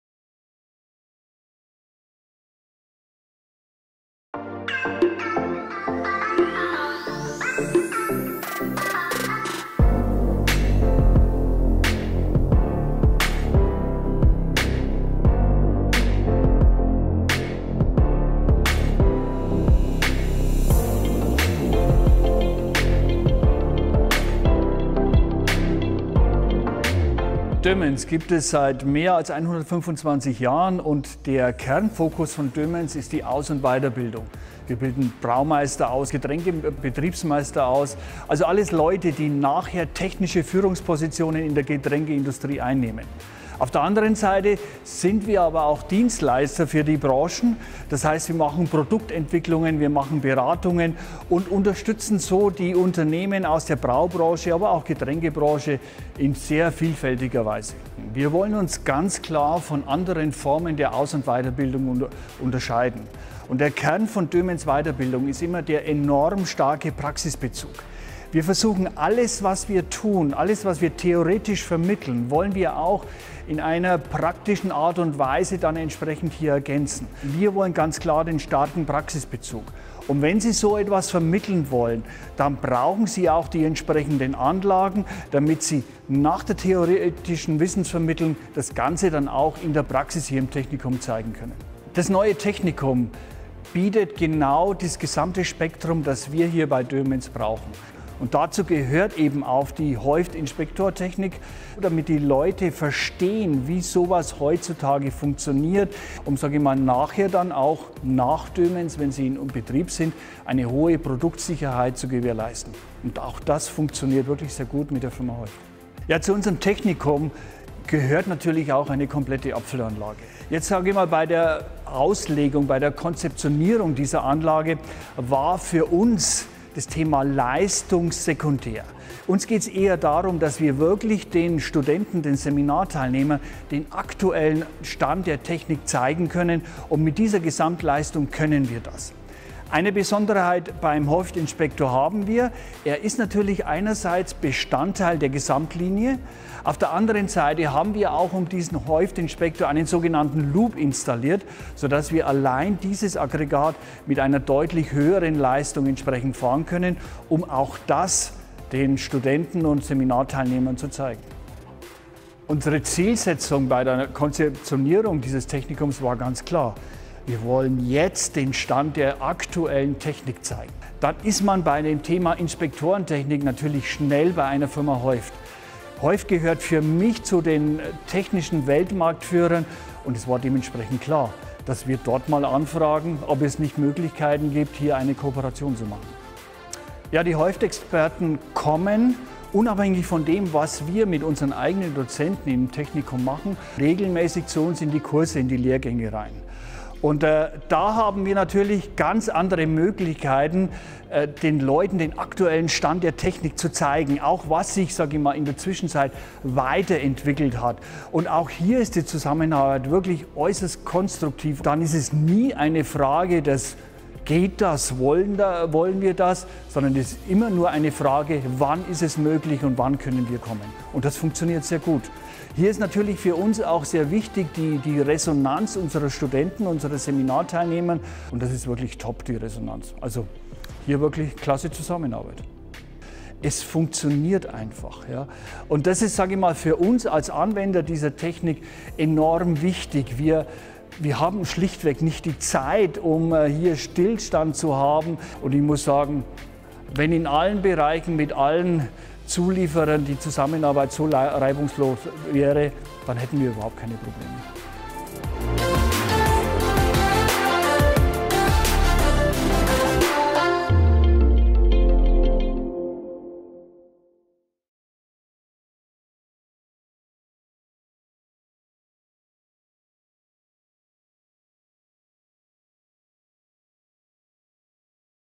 HEUFT bei Doemens: „Ein rundum gelungenes Projekt!“
Mit modernstem Equipment bietet das neue Abfüll- und Verpackungstechnikum der Doemens Academy in Gräfeling Studierenden der Brau-, Getränke- und Lebensmitteltechnik einen hohen Praxisbezug bei ihrer Ausbildung. HEUFT hatte die vor gut einem Jahr eingeweihte Anlage dazu kostenlos mit Kontroll-, Inspektions- und Flaschentransportsystemen ausgestattet. Ausführliche Infos über die erfolgreiche Zusammenarbeit und unser neues Doemens-Video in voller Länge gibt’s hier!